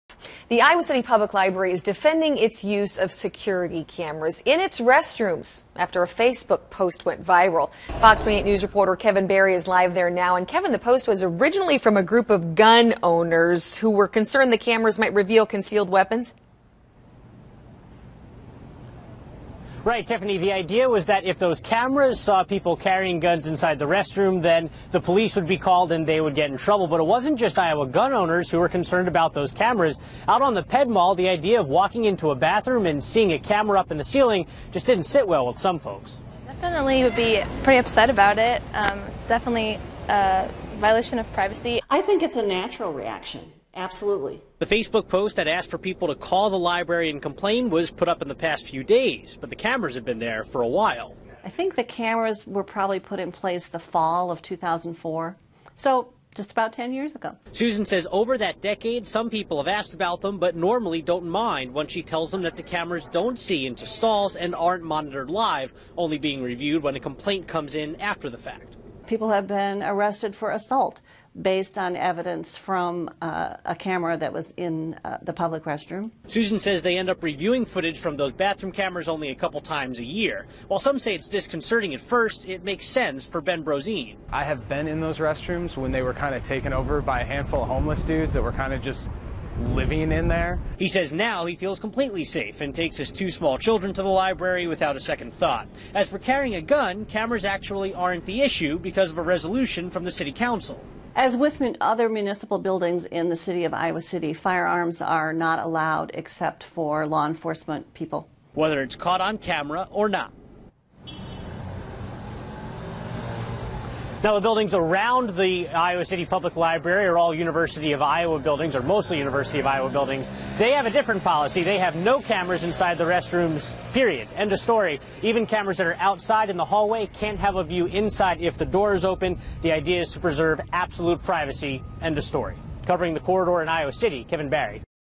Restroom Safety vs. Privacy
IOWA CITY, IA (CBS2/FOX28) -- The Iowa City Public Library is defending its use of security cameras in its restrooms after Facebook posts about them have been bringing in a lot of phone calls to the library.

The Iowa Gun Owners Facebook post says it's was worried about both privacy and concealed carry rights when they posted pictures of the cameras a few days ago.

Just talking to folks along the Ped Mall, the idea of walking into a restroom and seeing any camera just doesn't sit well.

"Definitely would be pretty upset about it," said University of Iowa junior Taylor Finch. "Definitively a violation of privacy."

"I think it's a natural reaction," said Iowa City Public Library Director Susan Craig.

The Facebook post that asks for people to call the library and complain was put up in the last few days, the cameras have been there for a while.

"I think the cameras were probably put in the fall of 2004, so just about 10 years ago," said Craig.

Susan says over that decade, some people have asked about them. They normally don't mind once she tells them that the cameras don't see into stalls, and aren't monitored live. The footage is only reviewed when a complaint comes in after the fact.

"People have been arrested for assault based on evidence from a camera that was in a public restroom," said Craig.

Susan says they end up reviewing footage from those bathroom cameras only a couple times a year.

While some say it's disconcerting at first, it makes sense for Ben Brozene.

"I have been in those restrooms when they were taken over by a handful of homeless dudes when they were kind of just living in there," said Brozene.

He says now,he feels completely safe, and takes his two small children to the library without a second thought.

As for carrying a gun, cameras aren't actually the issue because of a resolution from the Iowa City City Council.

"As with other municipal buildings in the city of Iowa City, firearms are not allowed except for law enforcement people," said Craig.

The rules of the road say, "no guns," whether they're caught on camera or not.

The University of Iowa has a different policy. They say they don't have any cameras in any university bathrooms and cameras outside bathrooms aren't even allowed to have a view inside through an open door.

The idea is to preserve absolute privacy in the restrooms.